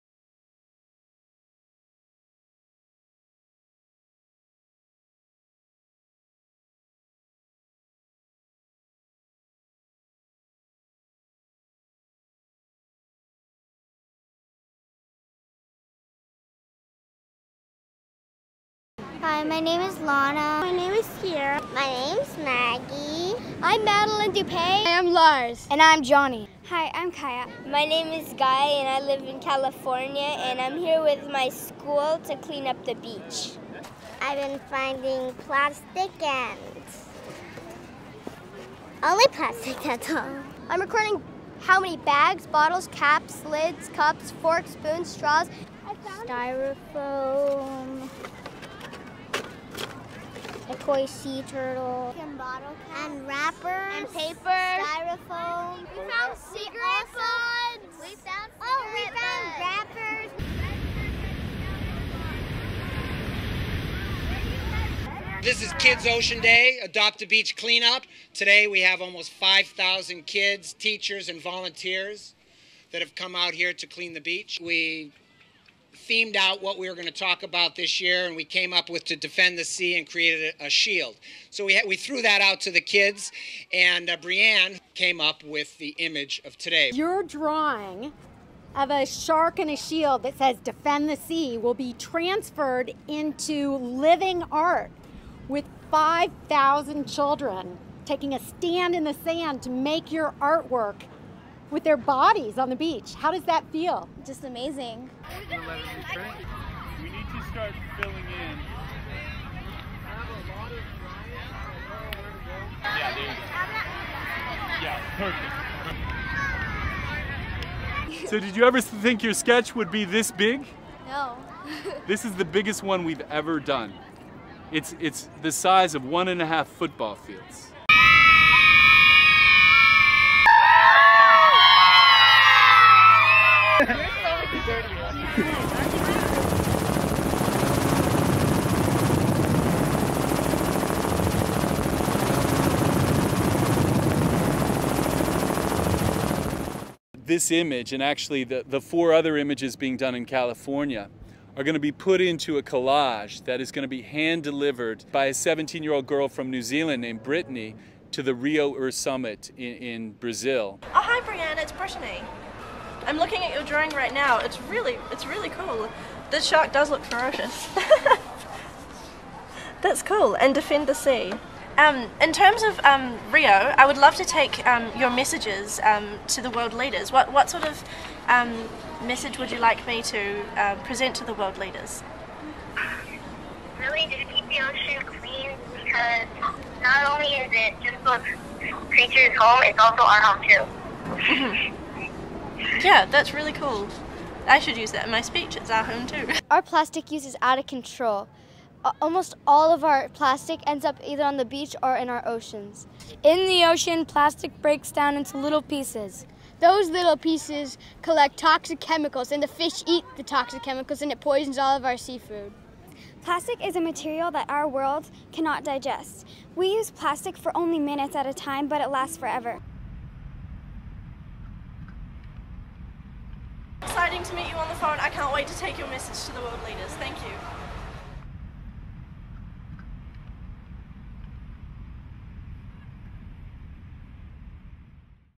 2012 KIDS OCEAN DAY TO RIO + 20 Earth Summit
Over 8,000 California KIDS in 5 cities form massive KID designed shields and marine
animals to say DEFEND THE SEA from everyday plastic pollution.
In Los Angeles, The Malibu Foundation for Environmental Education, the California
Coastal Commission, the City of Los Angeles, Keep Los Angeles Beautiful and Spectral Q organized the 19th Annual KIDS OCEAN DAY Adopt-A-Beach Clean-Up on June 7, 2012.

The kids are alerting the world about the need to protect the ocean from the everyday
trash and plastic litter that ows down streets, killing marine life and polluting food
resources.

Plastic in the ocean demands an international response. Spectral Q, Plastic Pollution
Coalition, and Natural Resources Defense Council (NRDC) has hand-carried delivery of
the aerial photographs made for KIDS OCEAN DAY, to Rio + 20 Earth Summit, where plastic pollution is on the international agenda.

LA Shark Designed by Breann Mancilla, Los Angeles 6th Grader/Photo by Lou Dematteis/Spectral Q/Malibu Foundation

San Diego—I Love A Clean
Orange County—Orange County Coastkeeper
San Francisco Bay Area—Richardson Bay Audubon
Humboldt County—Friends of the Dunes
State of California—California Coastal Commission